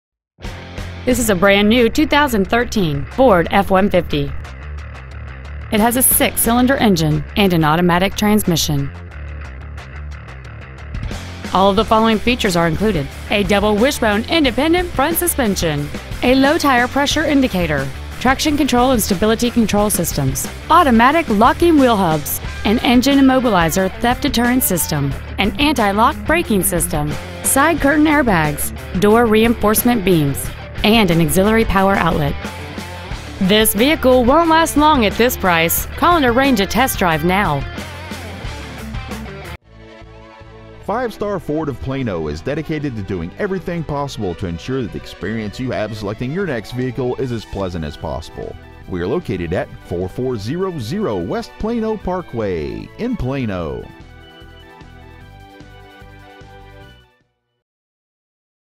2013 FORD F-150 Plano TX
Year : 2013
 Make : FORD
 Model : F-150
 Engine : 3.5L V6 24V GDI DOHC TWIN TURBO
 Trans . : 6-SPEED AUTOMATIC
 Exterior : BROWN
 Miles : 9
 Interior : PALE ADOBE
 Stock : DKD40921